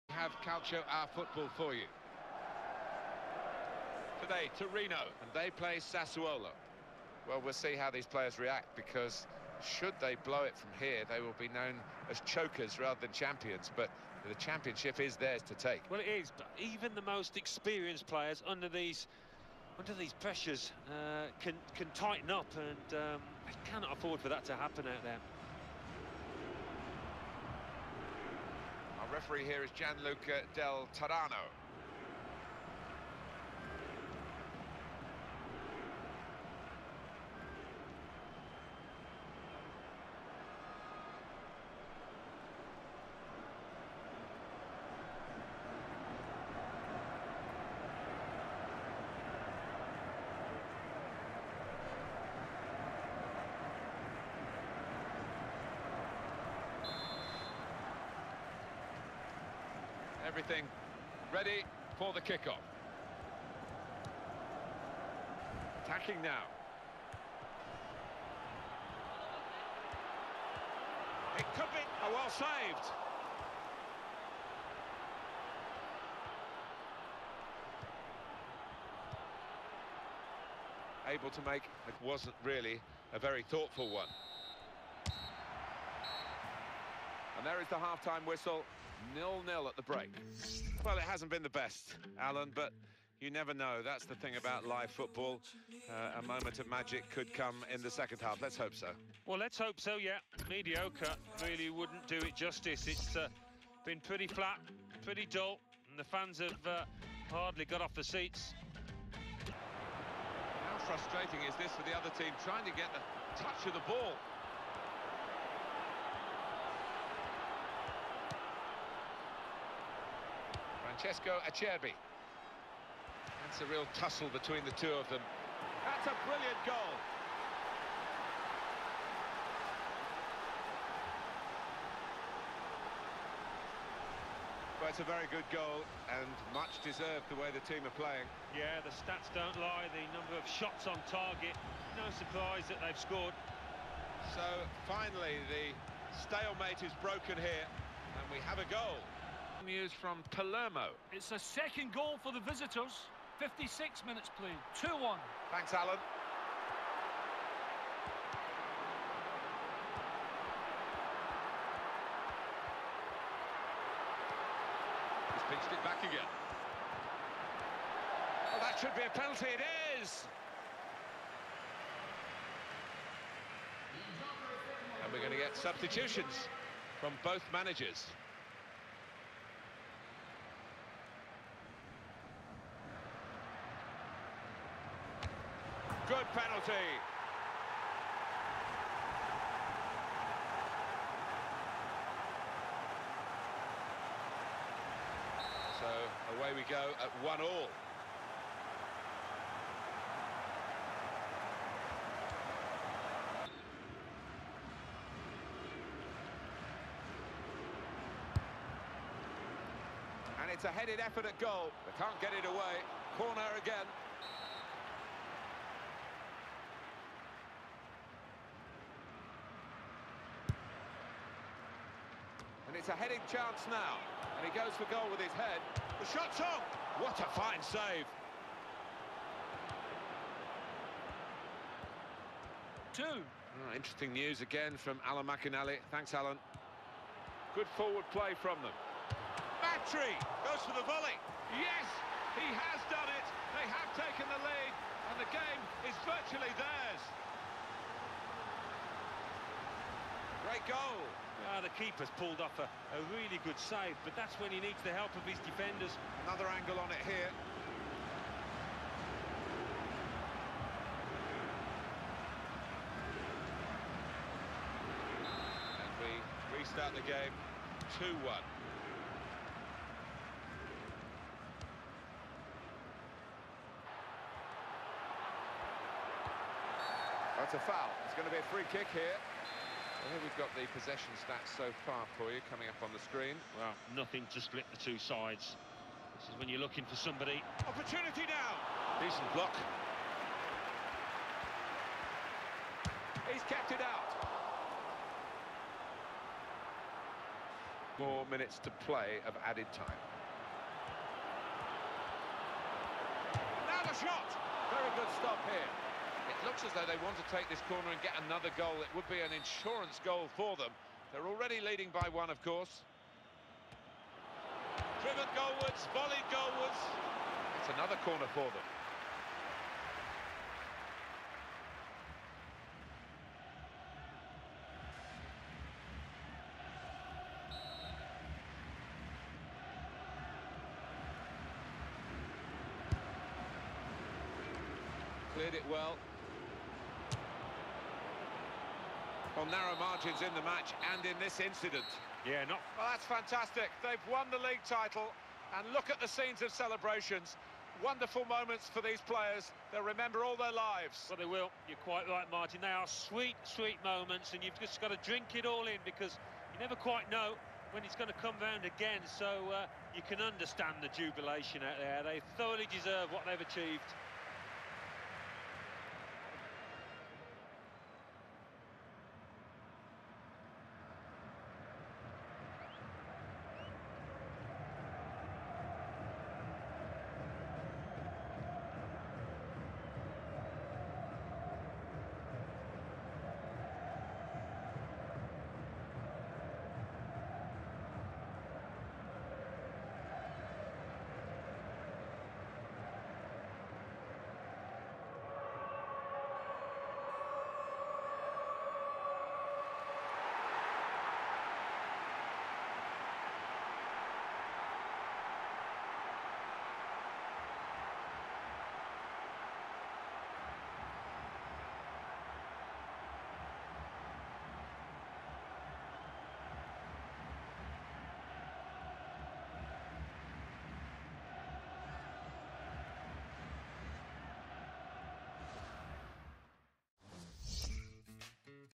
Late goal from Politano seals title for Sassuolo. (Serie A)
CAMPEONES!!!

Where should we go to next? Should we depart US Sassuolo in the summer or attempt to win the Champions League/Europa League with them?

My Top 5 Clubs We'd join for the next league: PSG (Ligue 1), Borussia Dortmund (Bundesliga) , FC Bayern (Bundesliga) , FC Barcelona (La Liga), Real Madrid (La Liga). I'd even consider a move to any English club.

DROP US A TWEET